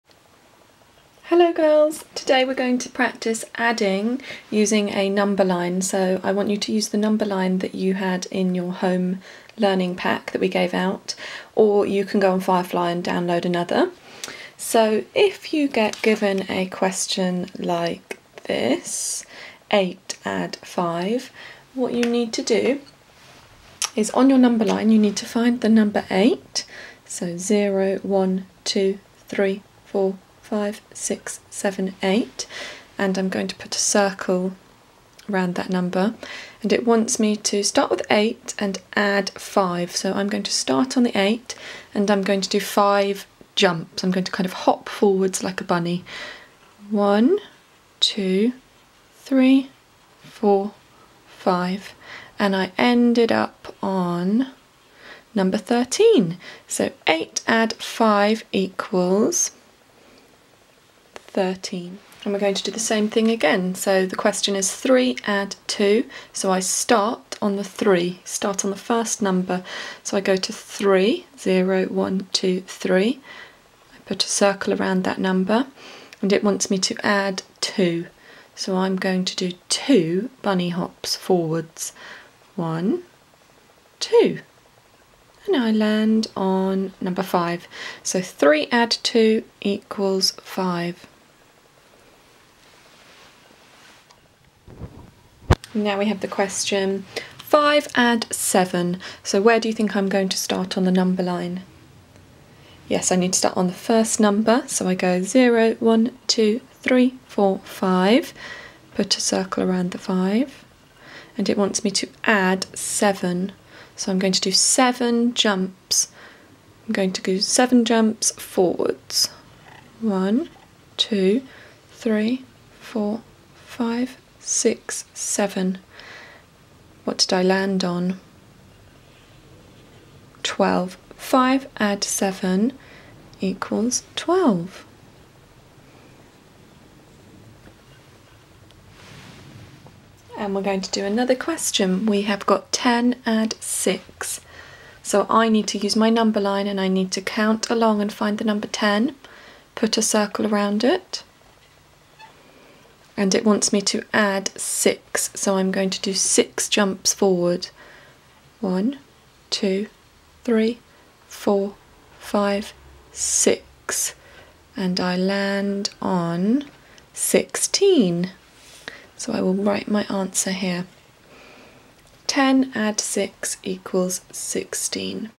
Adding by ‘counting on’ using a numberline
This video will help you practise adding by ‘counting on’.